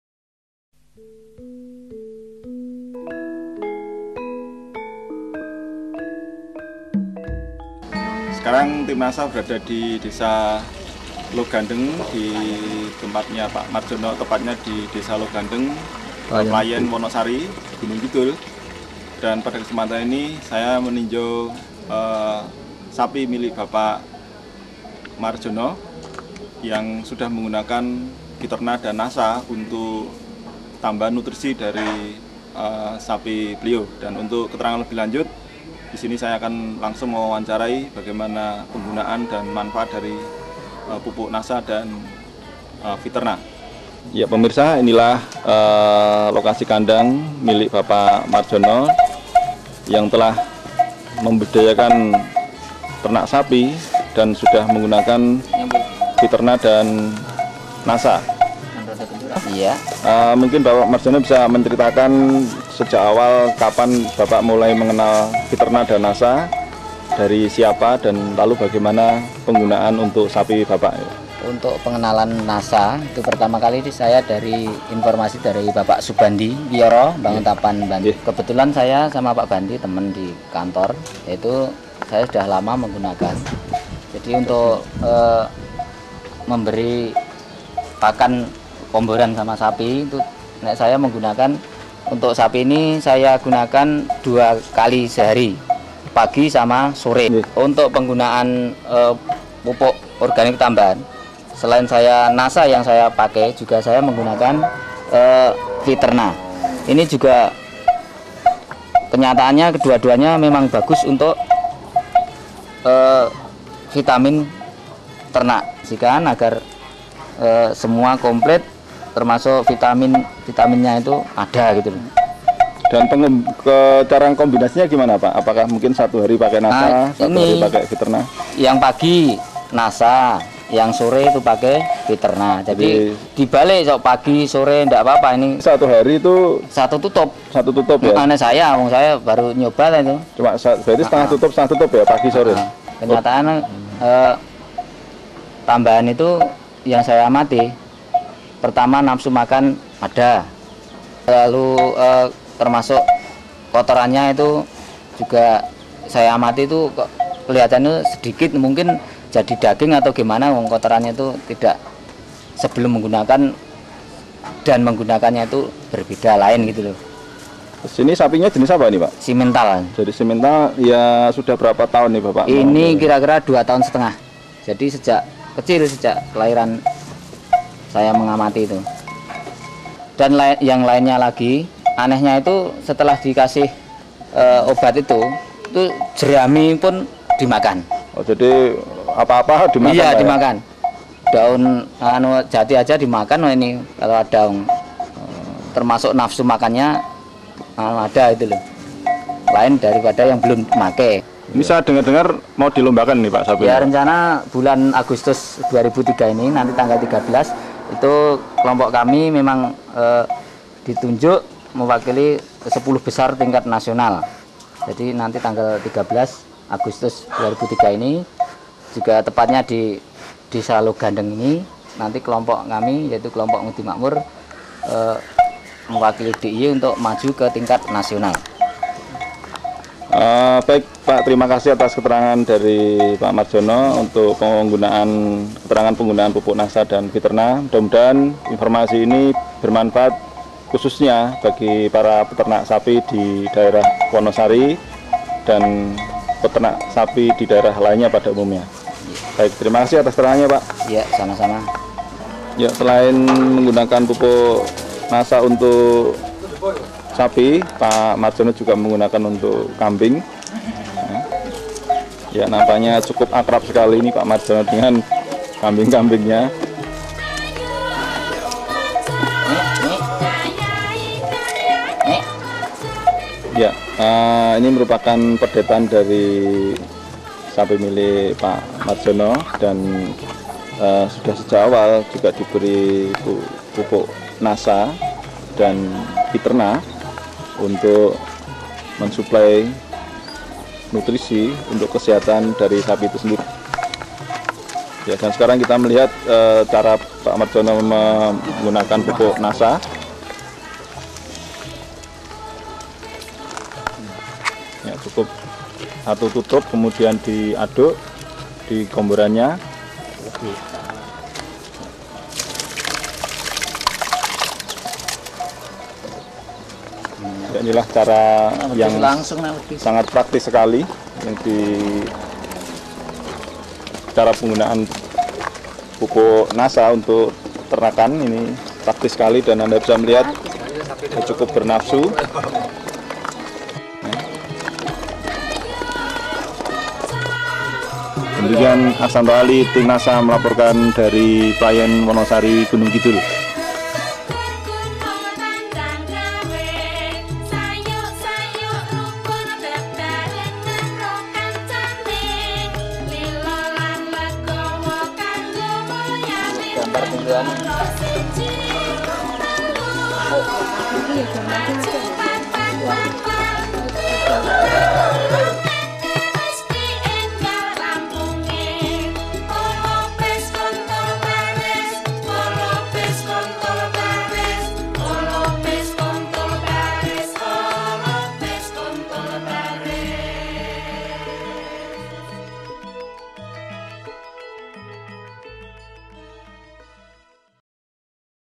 akibat pakai produk nasa SAPI KAMBING WONOSARI 08122777340
setelah pakai produk nasa perkembangan ternak sangat memuaskan..produk nasa memang luar biasa